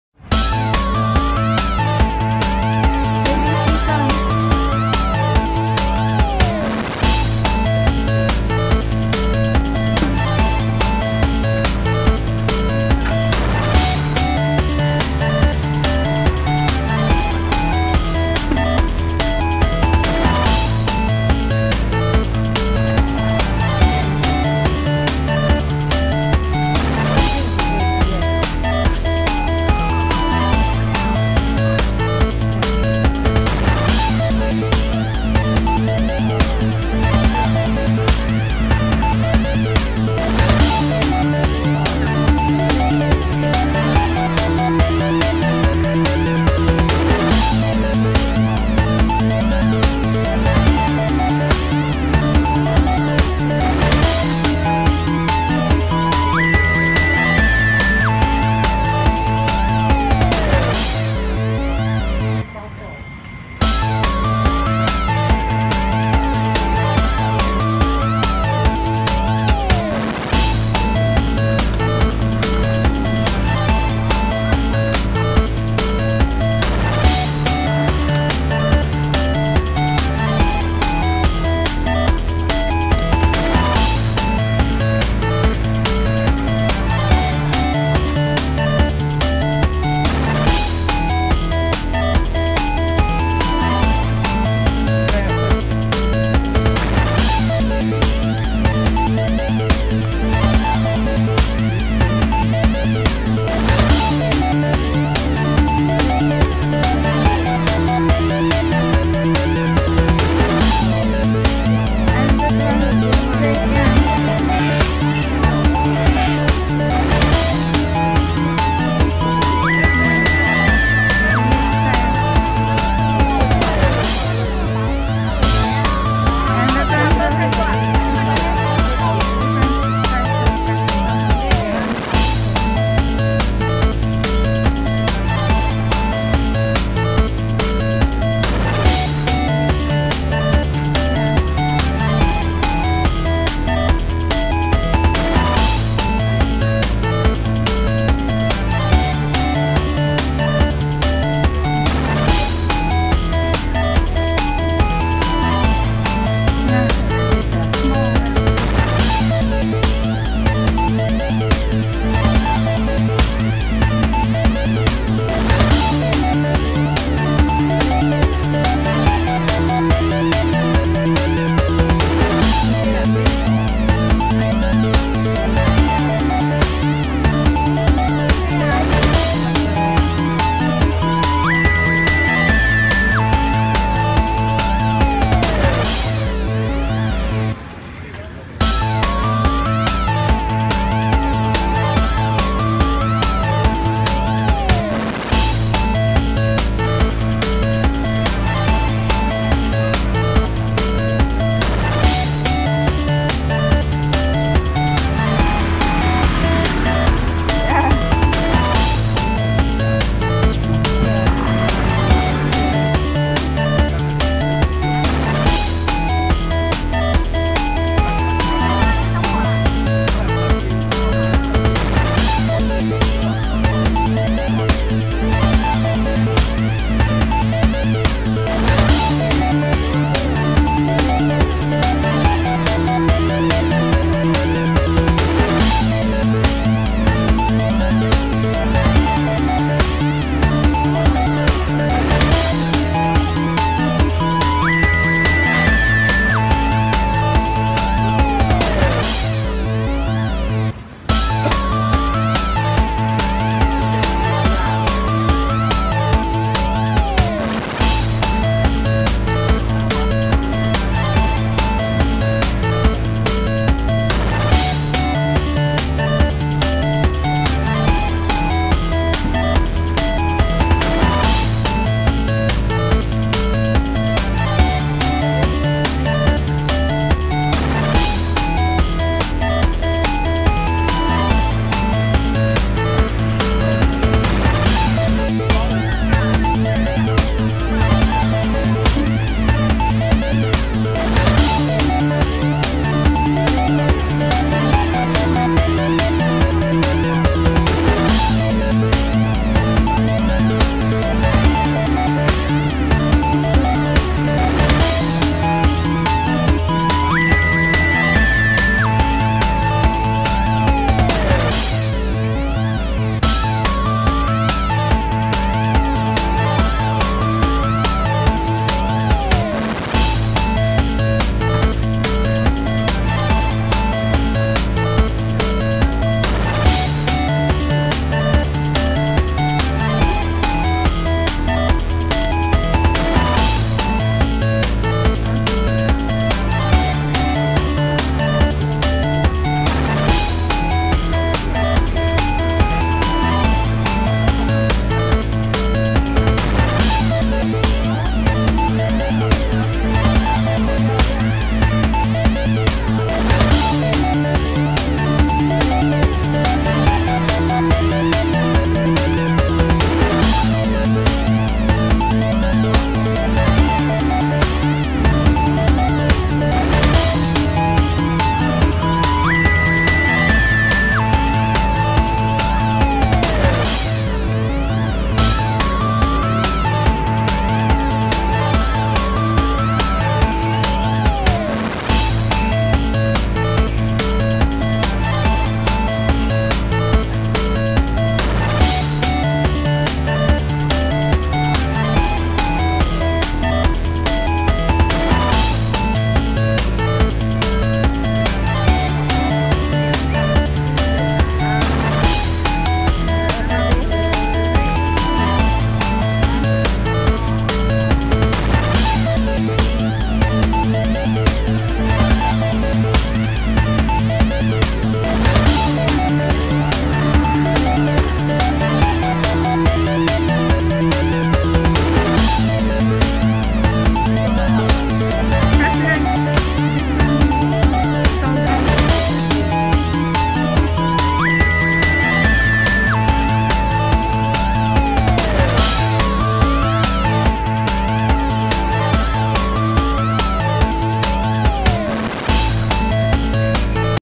Walking around Labor Park Flower Market
Just enjoying the flowers and the different kinds of plants and of course choosing some to buy🥰🥰🥰